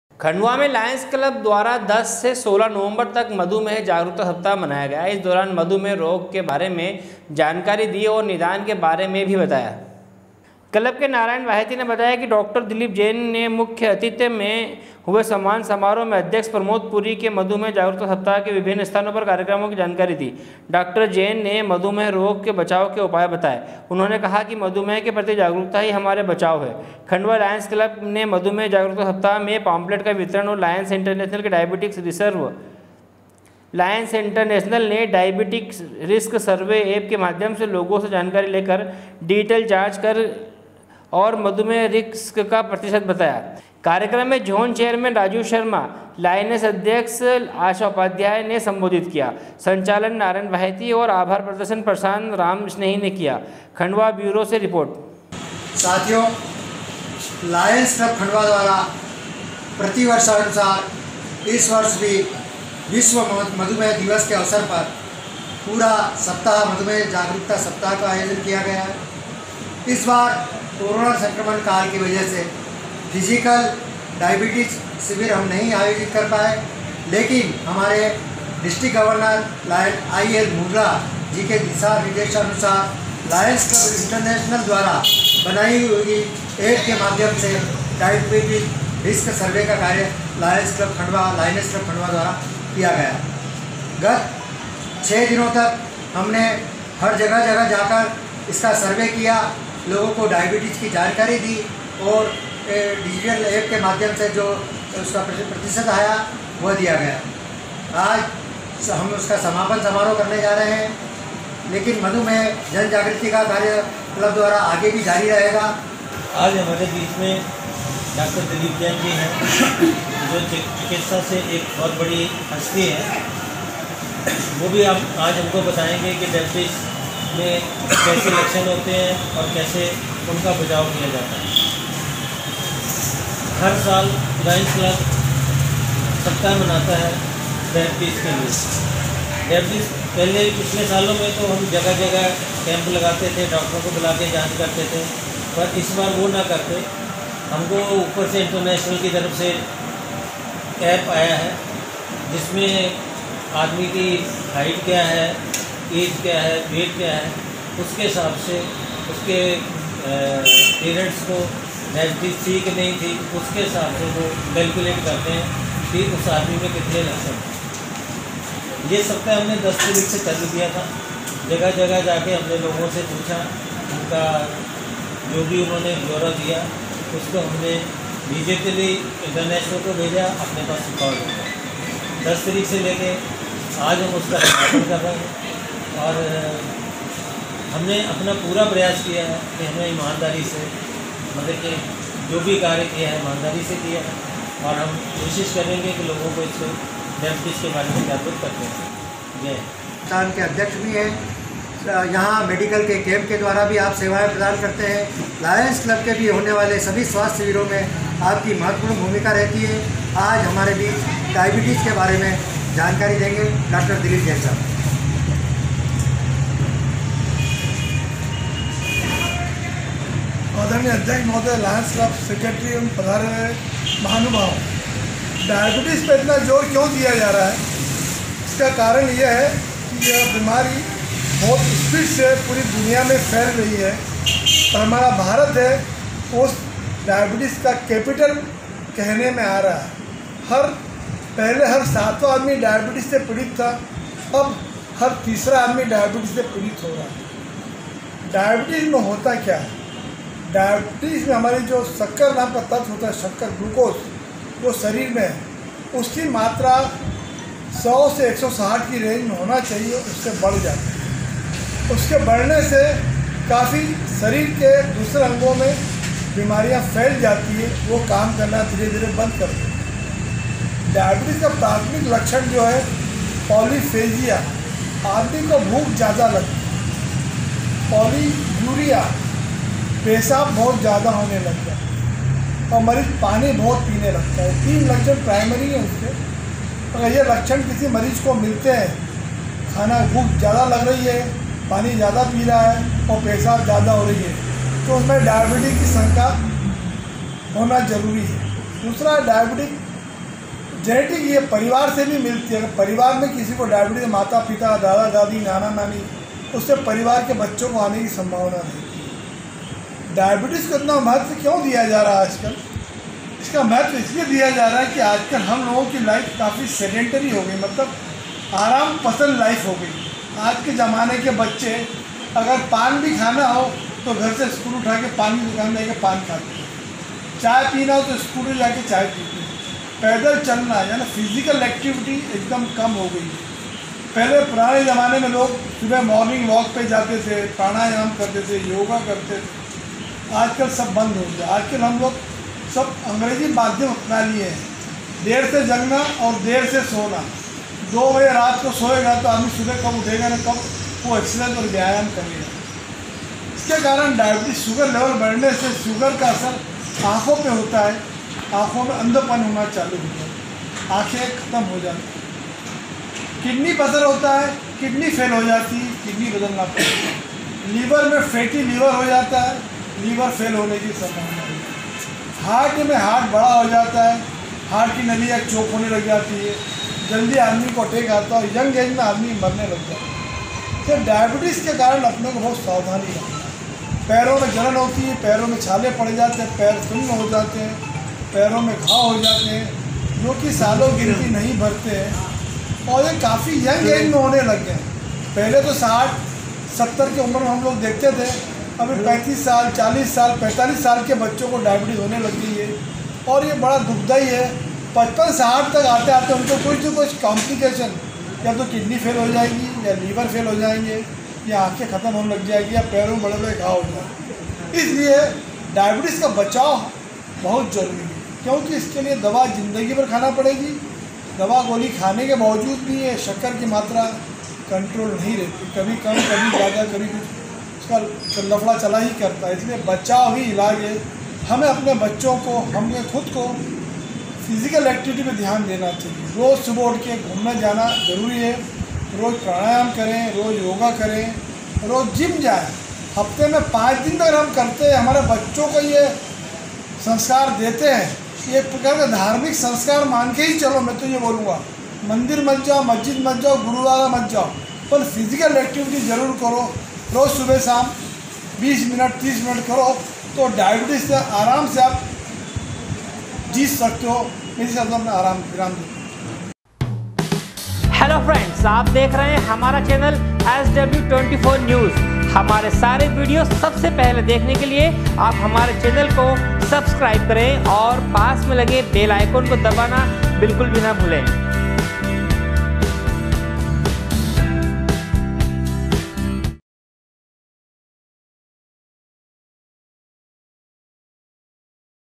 Khandwa | SW 24 News Madhyapradesh
खण्डवा में लायंस क्लब द्वारा 10 से 16 नवंबर तक मधुमेह जागरूकता सप्ताह मनाया गया। इस दौरान मधुमेह रोग के बारे में जानकारी दी और निदान के बारे में बताया गया।